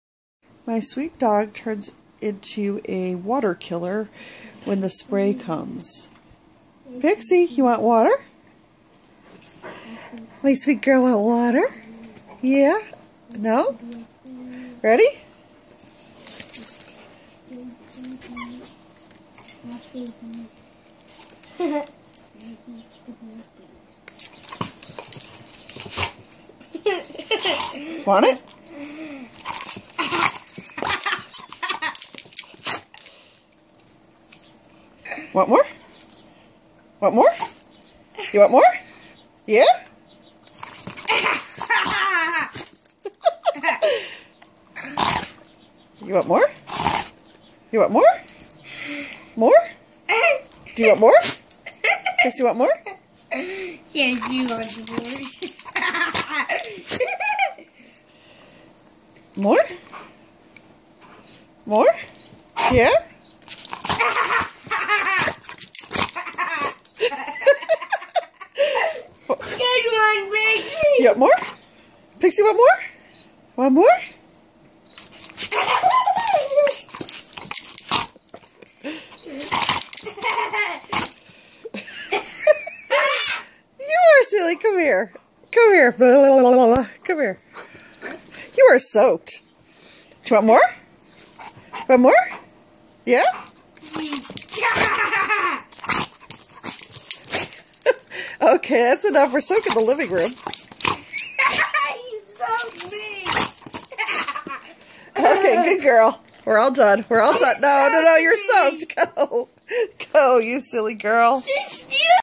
Pixie Dog Attacks Water
Our chocolate lab, Pixie, loves to get water squirted at her but she tends to look vicious and mean when she tries to bite the water.  She always does this weather it's the hose, a shower, a bath or just being squirted with a water bottle or squirt gun.